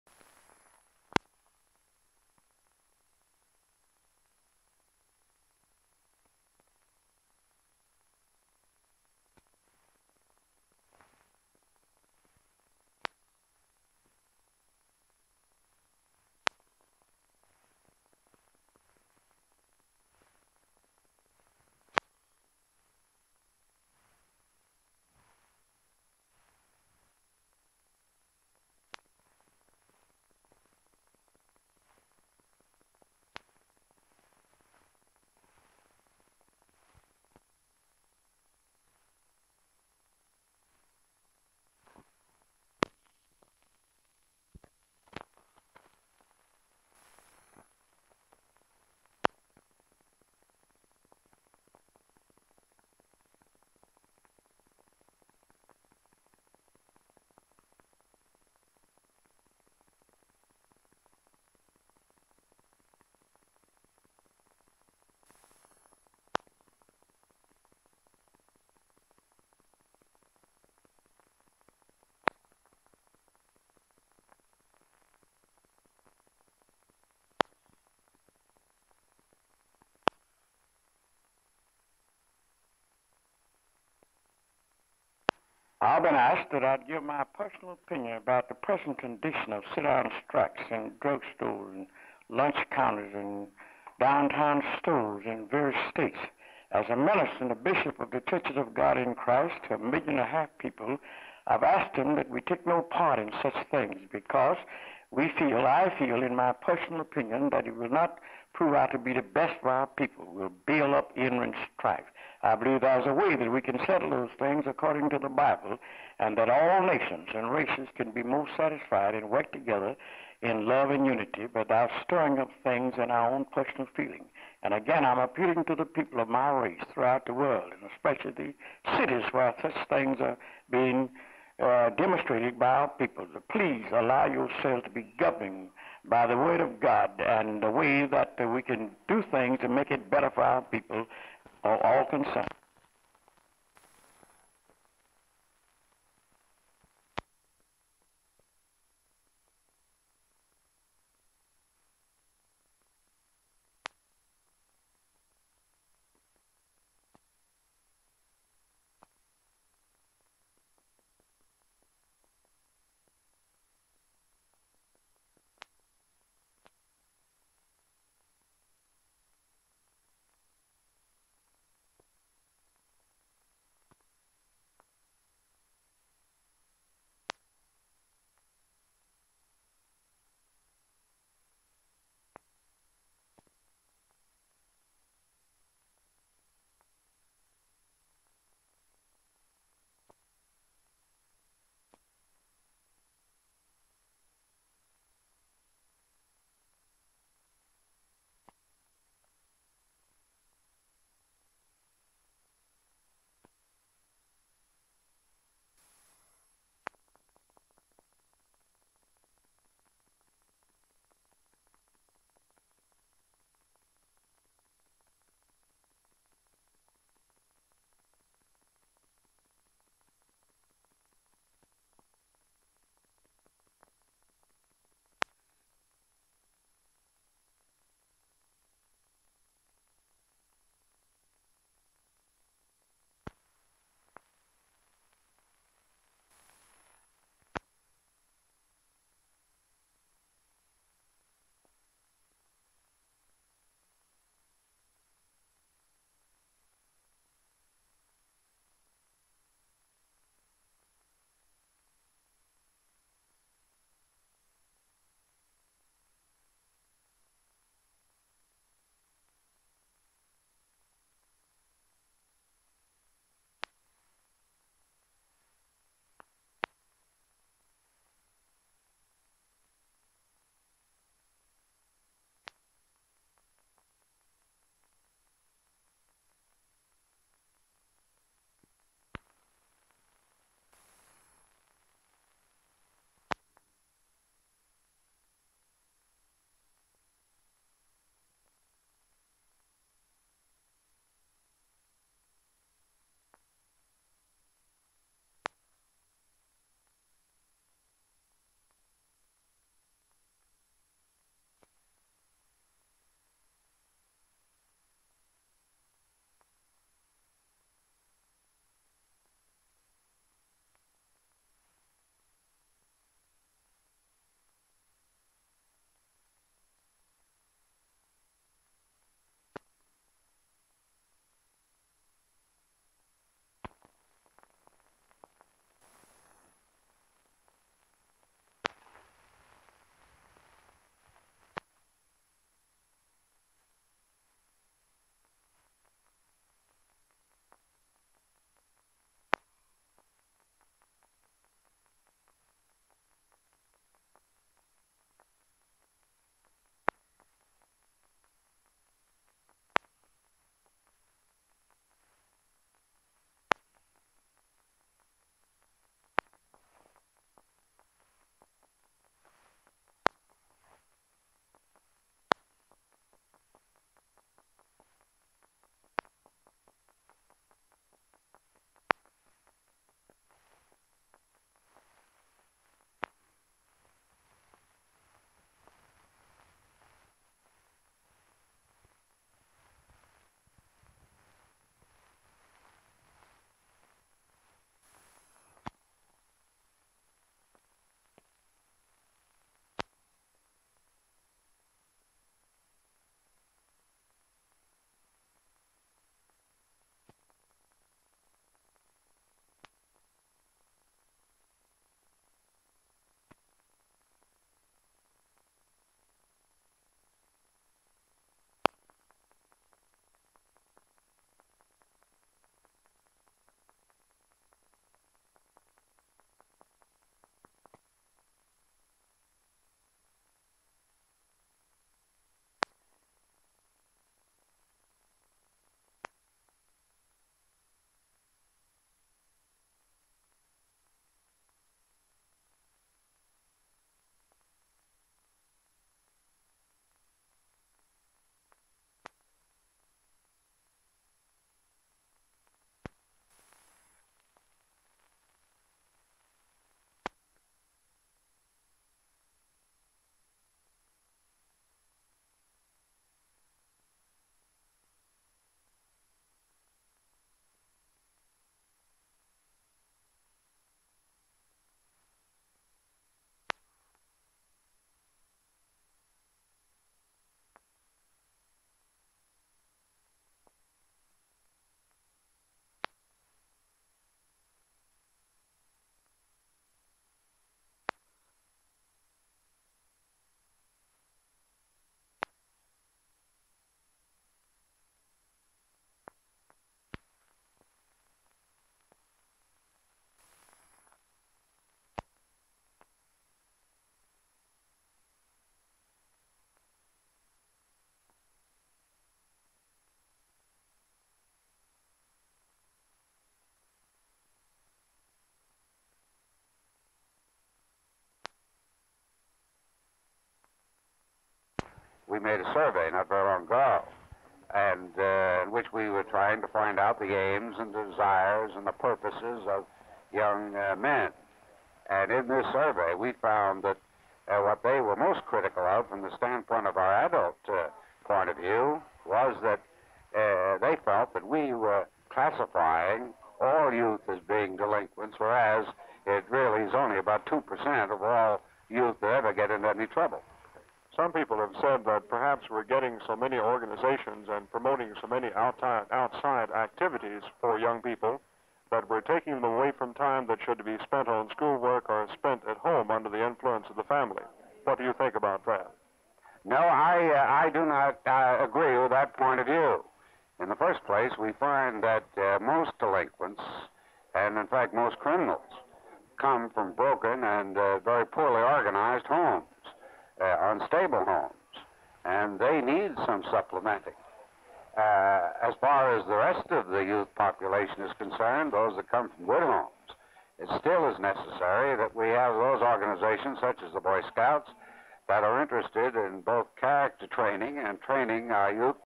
WFAA - April 22, 1960
0:00 - Aftermath of an automobile accident between a car and bus in Dallas, a fire hydrant is opened by a fireman to clean up spillage from the wreckage (Silent).

0:47 - An interview with a black bishop/minister who is opposed to lunch-counter sit-ins and demonstrations by African American activists; his remarks are prefaced by scenes of African American children enjoying lollipops.

2:26 - Dallas police investigate two separate robberies in Dallas: the first is in Pleasant Grove at Wrigley Food Super Market, 1339 S. Buckner where 4 armed and masked robbers got away with at least $7,000 after holding up the grocery's manager and locking the employees in the store's walk-in refrigerator; the second robber held up real estate man W. H. Sinclair in his car as he pulled up to his office at 1824 Record Crossing -- Sinclair was robbed of $3,000 and the bandit took his car, abandoning it in the Brook Hollow Industrial District (Silent).

3:56 - Scenes from a 2-alarm fire, involving 12 Dallas fire companies, at the Novelty Peanut Company located in a 3-story building at 1409 S. Ervay in The Cedars; footage includes firefighters battling the fire, people gathered near City Park watching, and a quick shot of the neighboring Ambassador Hotel; officials determined that the $10,000 fire sprang up spontaneously from sacks of oily peanut husks (Silent).

4:42 - Unknown meeting of several men in an office (Silent).

5:28 - The opening of Brookhaven Country Club's new clubhouse is celebrated with a week of events, including a performance by the Catalina Club's water ballet synchronized swimmers and a Titche-Goettinger swimsuit fashion show (with Titche's models walking on a ramp spanning one of the club's four swimming pools) (Silent).

6:28 - An investigation of a Dallas shooting in which 25-year-old John Joseph Thornton shot and killed his stepson, 24-year-old Odis Nail, in front of their home on or near Cedardale Rd., near Lancaster Rd. in far South Dallas. A body is taken away on a gurney, bullet holes are seen in an automobile, and police inspect a handgun (Silent).

7:25 - Four Texas representatives to the national 4-H Club conference in Washington, DC are honored at a luncheon at the Statler Hilton in Dallas; the delegates receiving framed certificates are Cinda Jane Ellis of Lancaster, Judy Dawson of Tulia, Donald Neumann of Chilton, and Pete Plank of Hereford (Silent).

8:11 - An interview with Dr. Arthur A. Schuck, CEO of the Boy Scouts of America, on his survey of the concerns of young men, a possible problem with outside activities, and the benefits of the Boy Scouts.